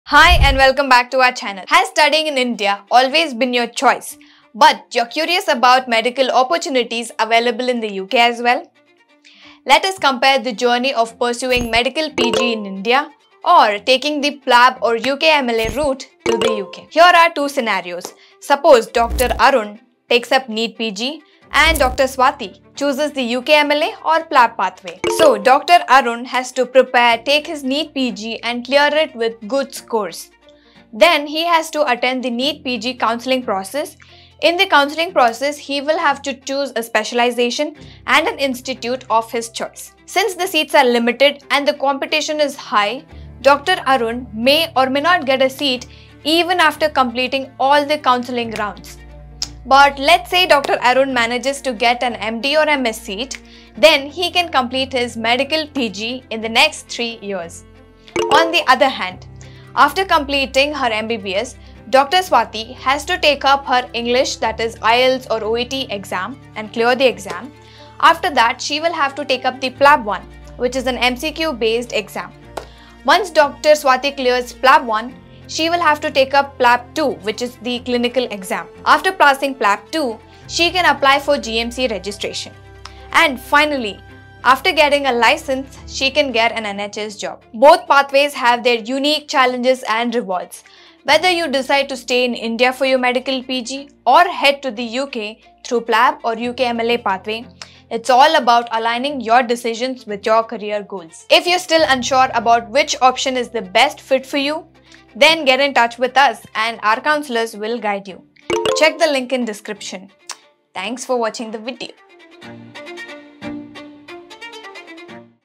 Medical PG in India vs UK (PLAB/UKMLA) | Which is better for your career?
Are you considering various options for pursuing a medical postgraduate degree? In this video, we compare the journey of pursuing a Medical PG in India and taking the PLAB/ UKMLA route in the UK.
Both pathways have their challenges and opportunities. Whether you opt for a Medical PG in India or pursue the PLAB/UKMLA pathway in the UK, your decision should align with your career aspirations.